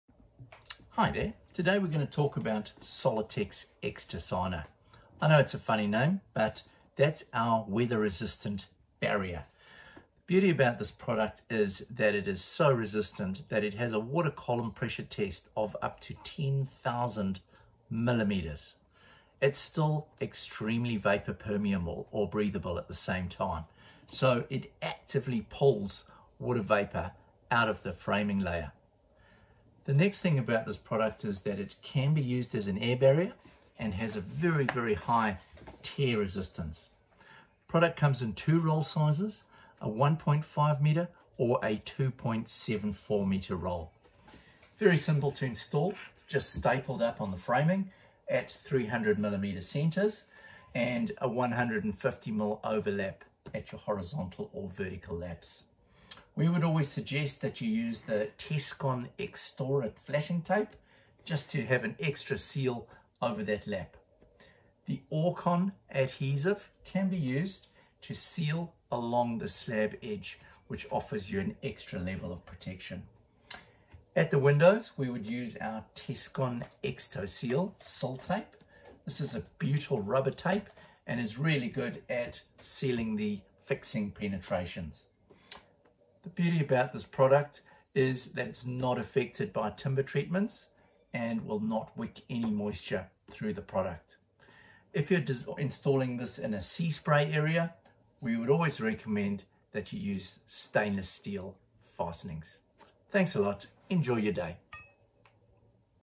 pro clima SOLITEX EXTASANA® Wall Weather Resistive Barrier
pro clima SOLITEX EXTASANA® is a high performance Weather Resistive Barrier (WRB). It's a non-porous, breathable membrane with a 10,000 mm water column pressure resistance. This means it can withstand a vertical column of 10 m of water without leaking, yet it's vapour permeable. So it not only prevents your building from getting wet, it allows it to dry out too.

Key features and specifications:
- Vapour resistance: 0.75 MNs/g
- Can be used as an air barrier
- High tear resistance
- UV resistance: 90 days
- Roll sizes: 1.5m or 2.74m

Installation:
- Lap 150 mm (to printed marking on the material)
- Staple at 300 mm centres
- Seaspray zone, use stainless steel fasteners